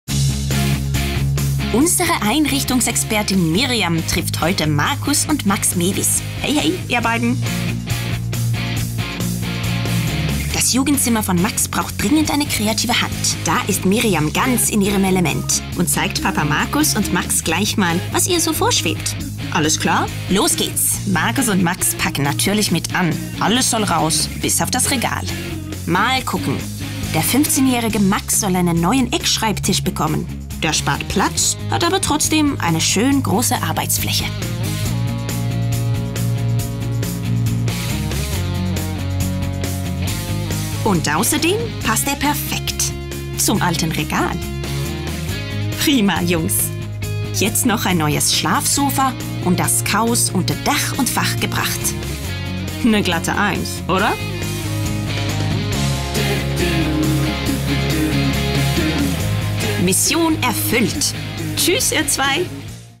IKEA verwirklicht Ideen: Mission Jugendzimmer
Max braucht mehr Platz zum Lernen. Deshalb hat unsere IKEA Einrichtungsexpertin einen cleveren Schreibtisch mitgebracht. Und sogar ein neues Sofa dazu.